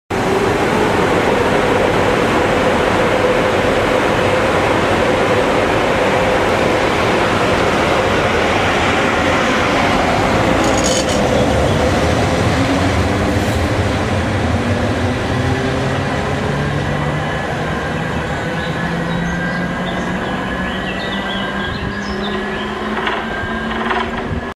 WANDSWORTH ROAD 73107 SPITFIRE 28 5 2010
Juat after i packed up and was half way over the bridge this 73 arrived on the back ran back over the bridge and shot a small piece. good day for eds.